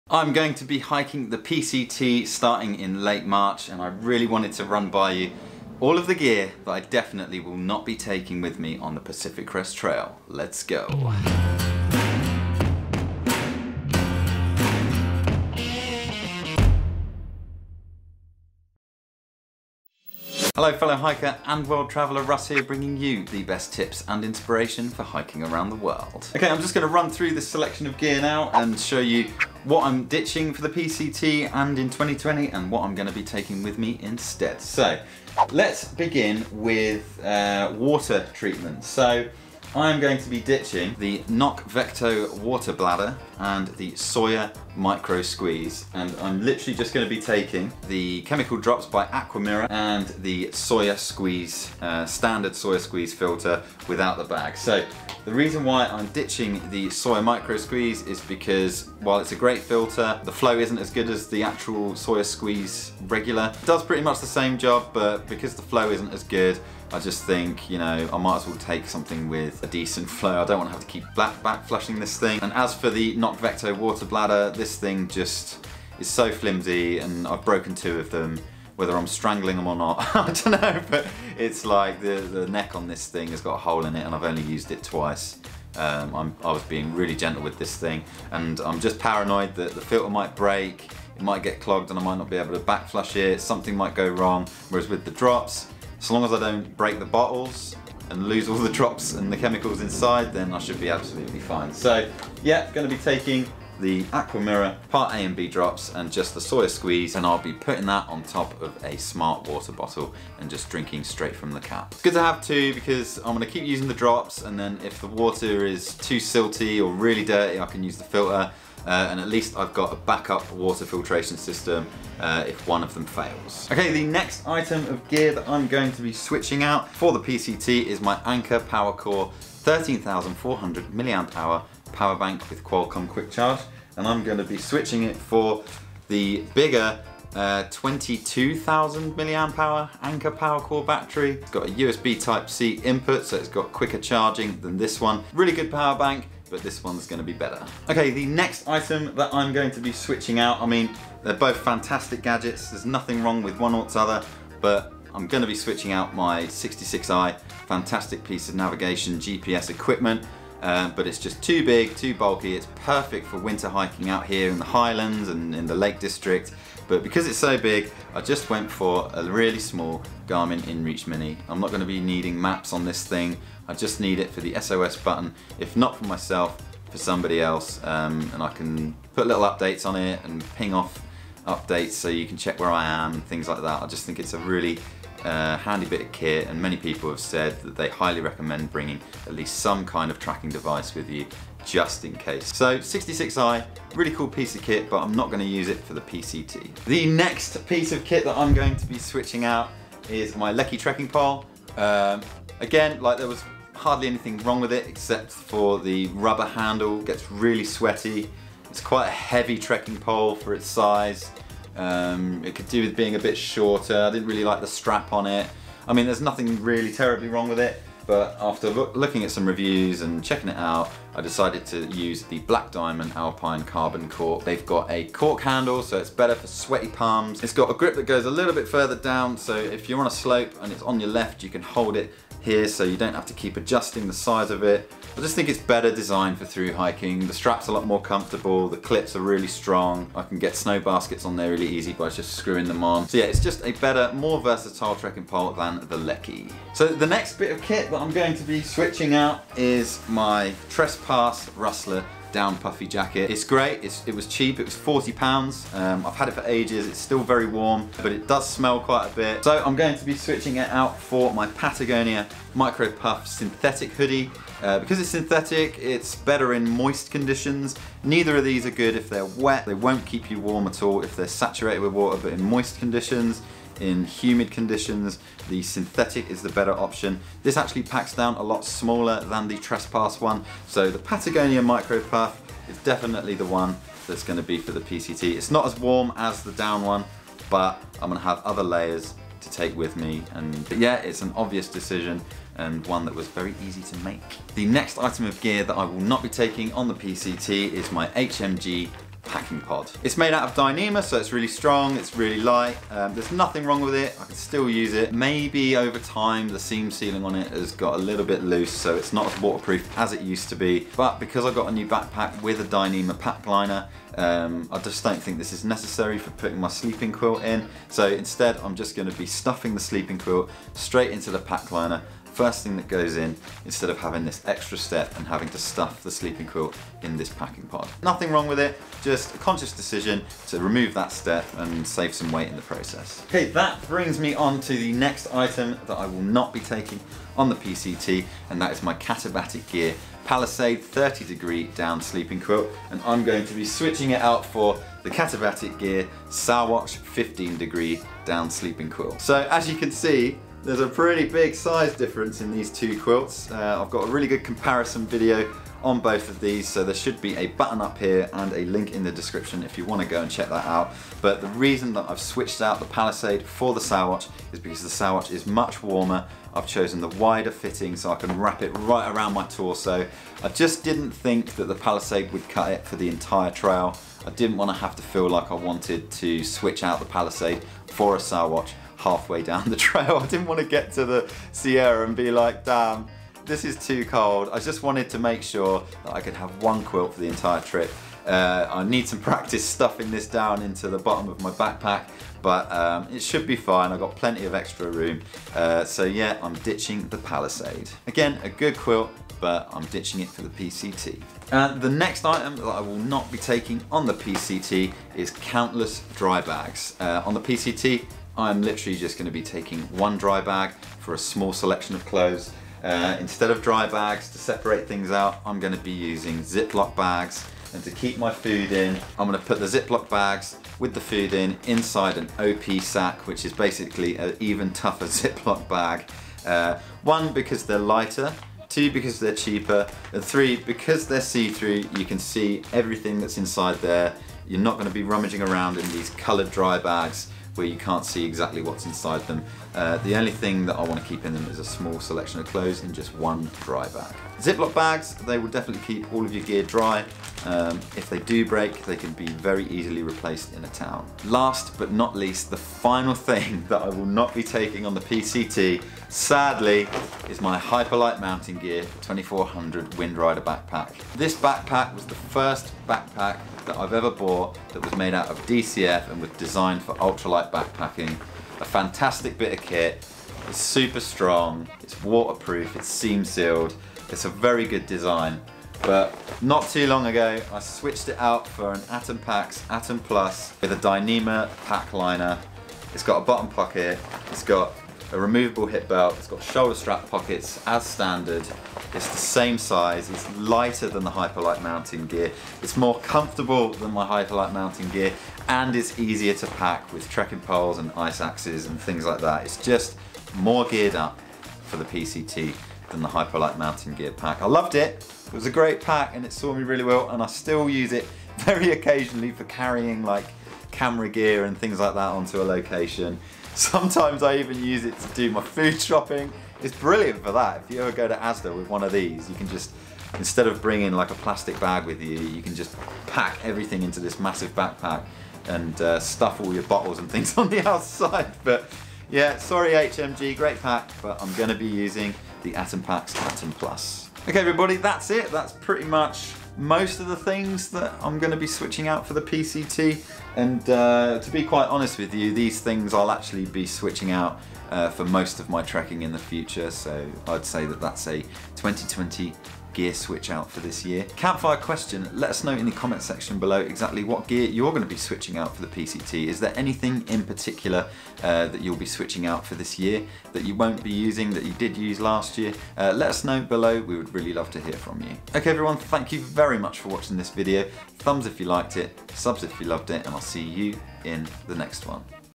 Gear I WON'T be taking on my 2020 Pacific Crest Trail thru-hike
Still dialling in your hiking gear for the PCT? Here's most of the things I'm switching out for my 2020 PCT thru-hike to help you decide what to pack.

I won't be taking these things because they're either too heavy, not warm enough or just not needed. What's in my pack and gear list will differ to yours, but I hope it helps you decide what to leave at home for your thru-hike of the PCT this 2020.

My pack's base weight is around 14lbs (6.44kg) so it's almost in the Ultralight category so I'm still dialling it in.

CLOTHING

FILM AND SOUND

NAVIGATION

WATER TREATMENT

ELECTRONICS

EQUIPMENT

STORAGE